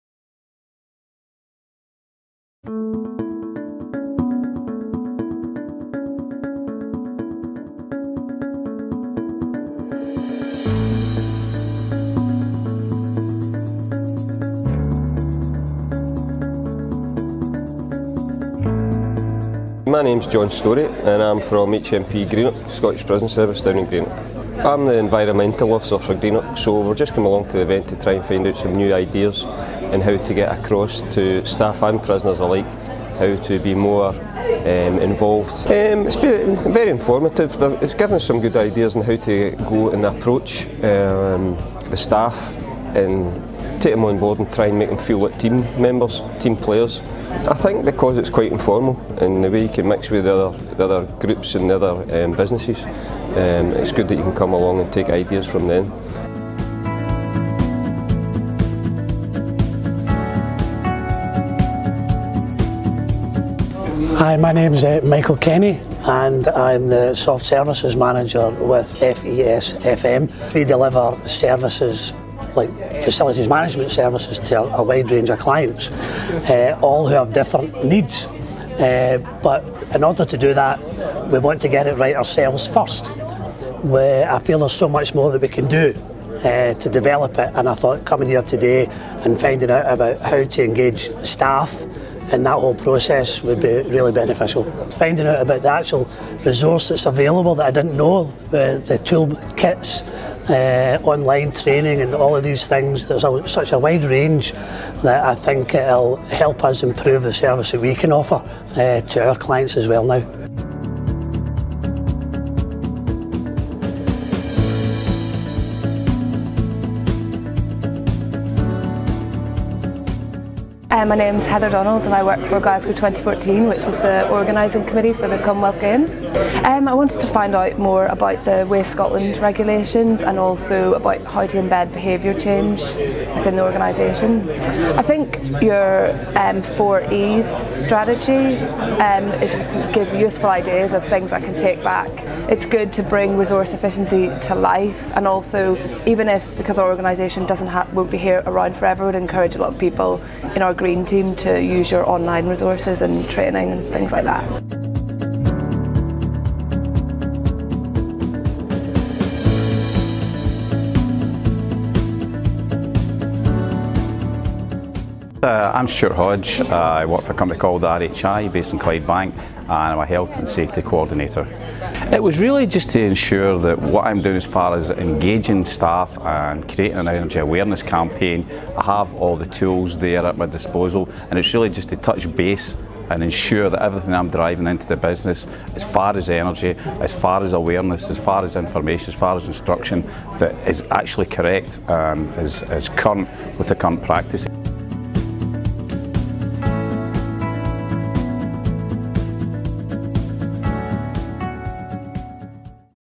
Workshop Highlights How to engage staff on resource efficiency HD
Highlights from our FREE half day workshop at Hampden Park Stadium, Glasgow.